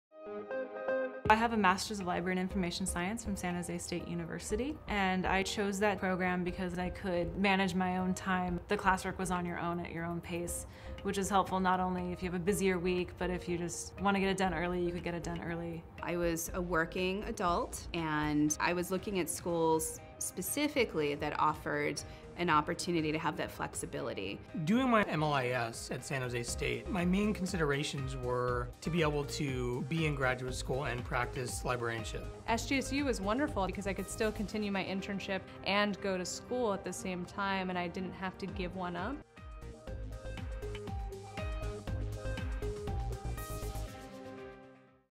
Why Get Your MLIS Degree from SJSU iSchool
Alumni share why they chose the SJSU School of Information's MLIS program.